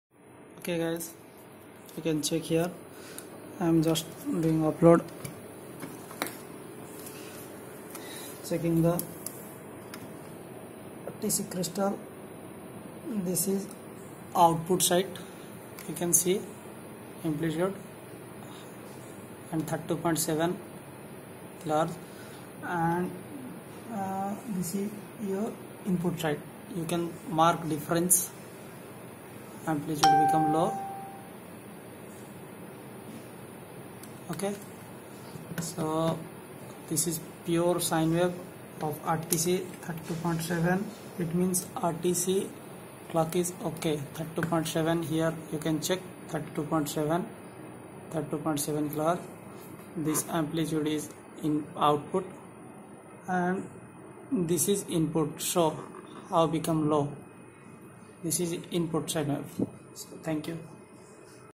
RTC TESTING BY DSO
Rtc sign wave testing in dso.